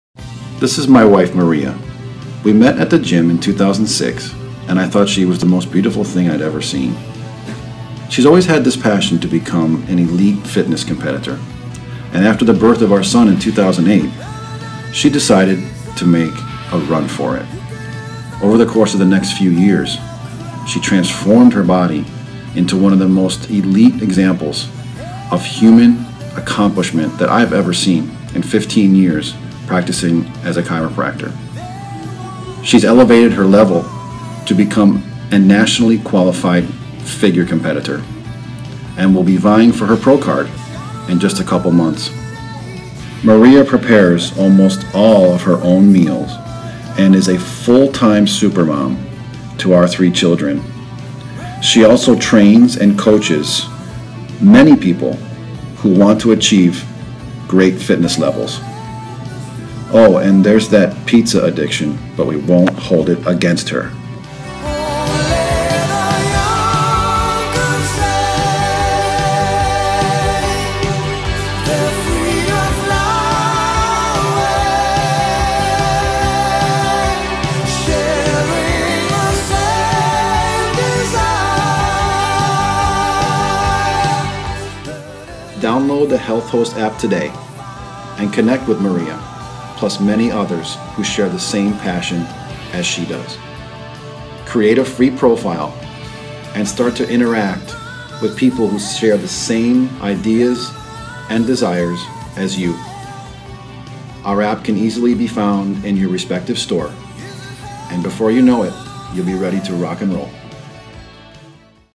Healthost Transformation: Maria Ruszkowski
Healthost Ambassador - Maria Ruszkowski's transformation is headlined in this Healthost.com production.